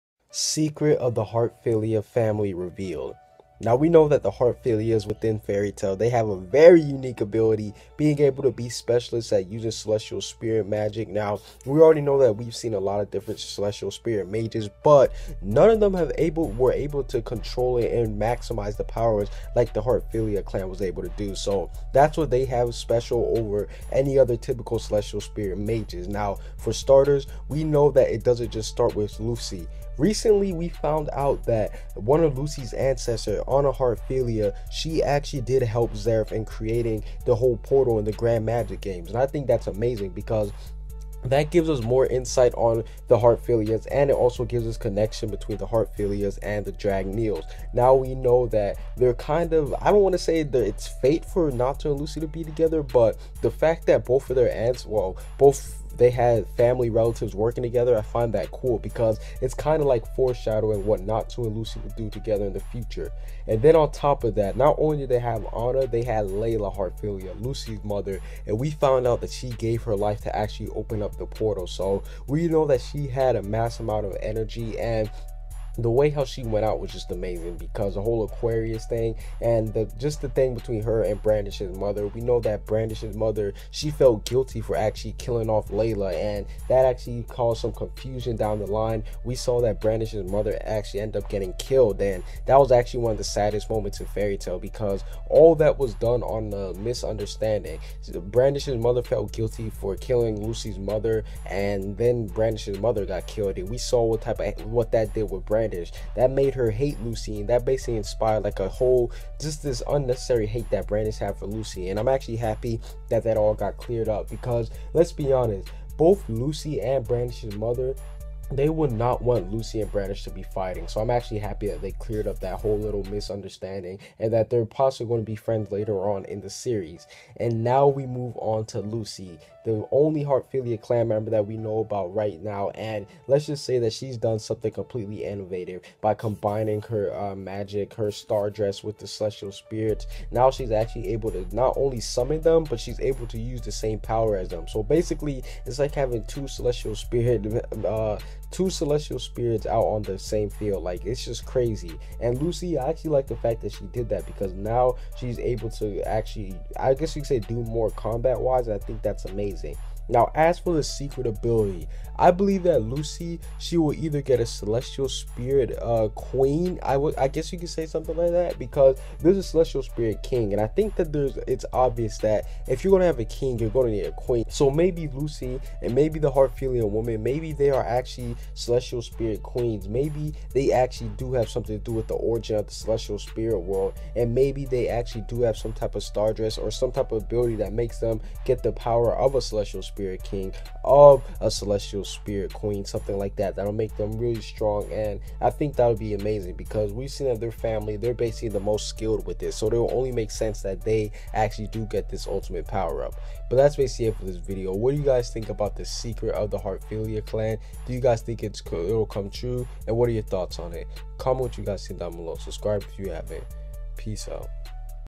Fairy Tail Secret Of The Heartfilia Family Revealed
Fairy Tail Chapter 495 - Hearfilia Family, & Anna, Layla, & Lucy Secret Ability Revealed!

Fairy Tail Chapter 494 - Dragneel Brothers, & Dragneel Family. Natsu Dragneel, Zeref Dragneel, & Larcade Dragneel Biggest Secret revealed Fairy Tail Episode .

In Fairy Tail Chapter 495 I discuss Lucy Heartfillia's Potential final ability in the series! Are you excited for Fairy Tail Chapter 495 & Episode 278?